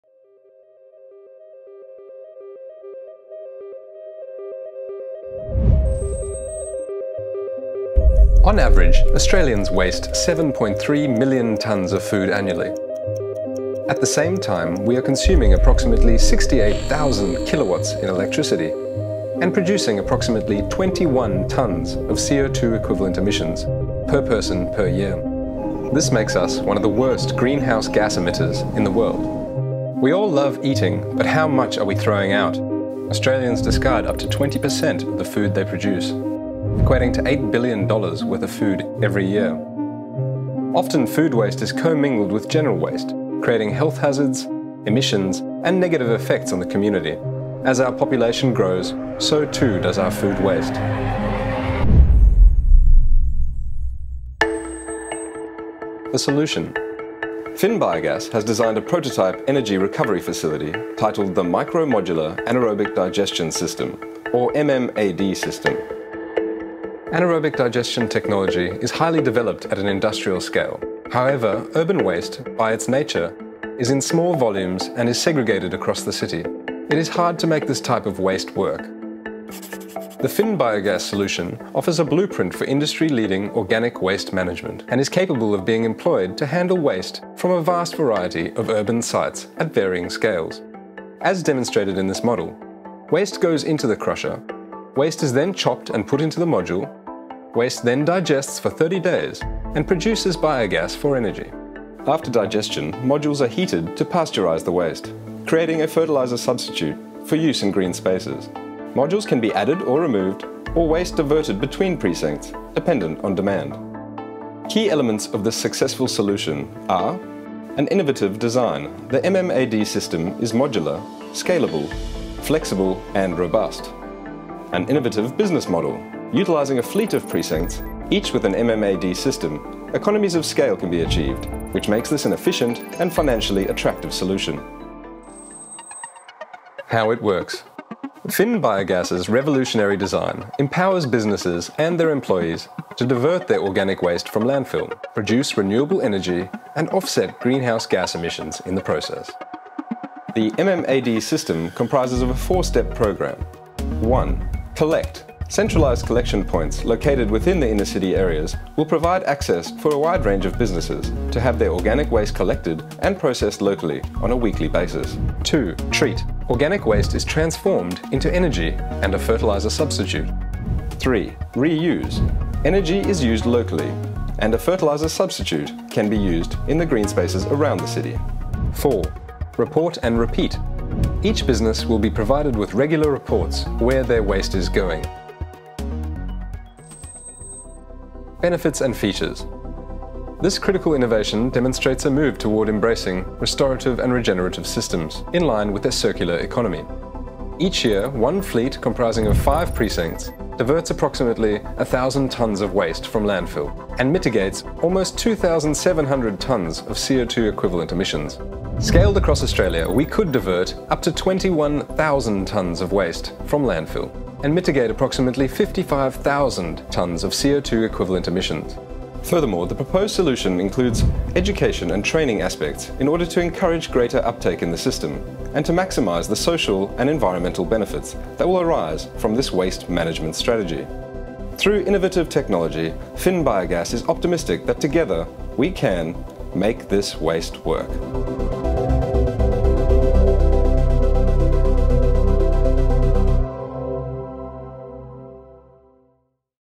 Micro Modular Anaerobic Digester (MMAD) System.
Finn Biogas will soon transform the way organic waste is disposed, thanks to a $1 million grant as part of the Federal Government’s @Business.gov.au Research and Innovation Initiative (BRII).

Our Micro Modular Anaerobic Digester (MMAD) System will empower businesses and their employees to divert waste from landfill, turning organic waste into energy and nutrient-rich fertiliser which would have otherwise gone to waste.

“Through innovative technology, we are optimistic that together we can make waste work and embrace a brighter future and circular economy through restorative and regenerative energy.” – Founder and Managing Director Jason Hawley.